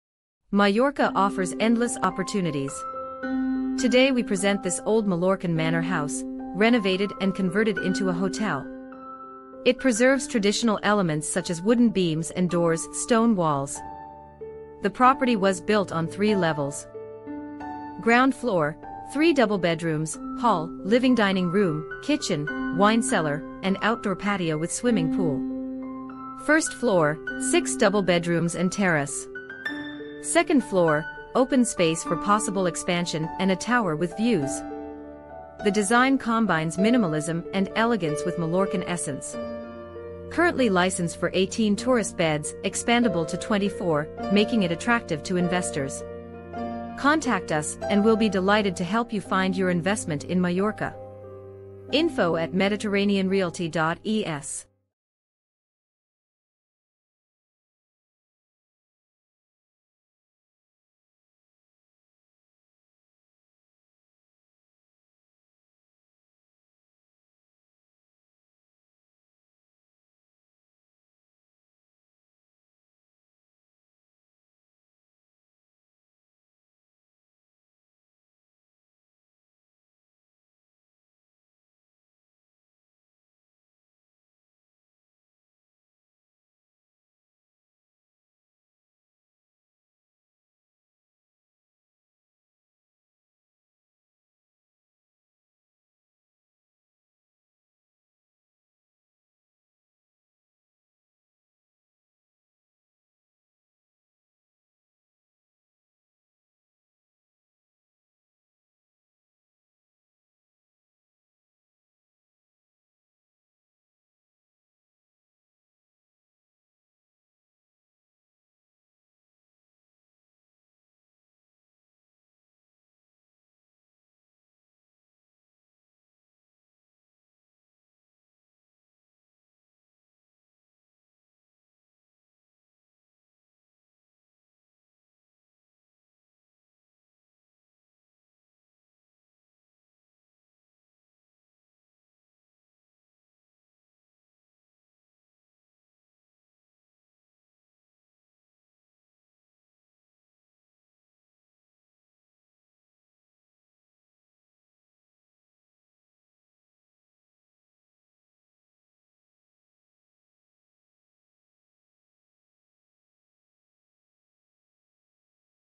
The Hotel for Sale that's Too Good to be True
REF. sohotsin

Mallorca ofrece un sin fin de oportunidades.
      Hoy les presentamos esta antigua casa señorial mallorquina, rehabilitada y convertida en hotel.
      Conserva elementos tradicionales tales como vigas y puertas de madera, paredes de piedra .
      Tiene  tres plantas:
    • Planta baja: 3 habitaciones dobles, hall, salón-comedor, cocina, bodega y patio exterior con piscina.
    • Primera planta: 6 habitaciones dobles y terraza.
    • Segunda planta: espacio diáfano para posible ampliación y torreón con vistas.
El diseño combina minimalismo y elegancia con esencia mallorquina. Actualmente tiene licencia para 18 plazas turísticas, ampliable a 24, lo que lo hace atractivo para inversores.